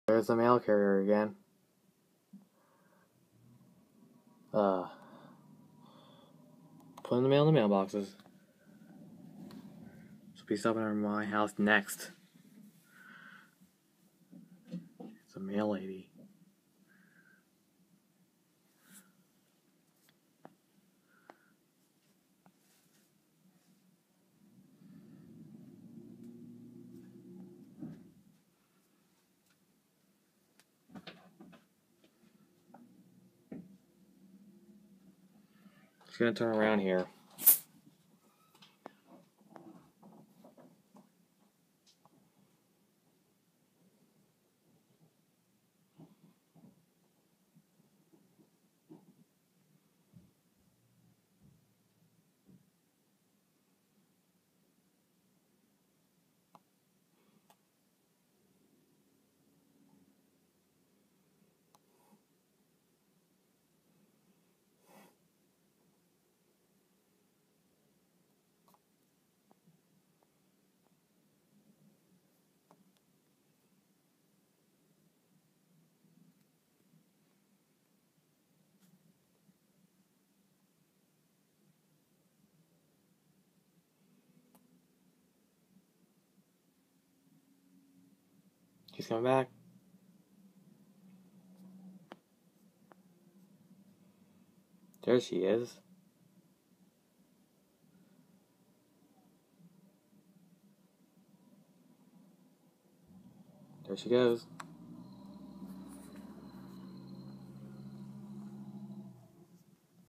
Mail Carrier - 12/22/14
Here is a video of the mail carrier in my neighborhood on Monday, December 22, 2014. Enjoy!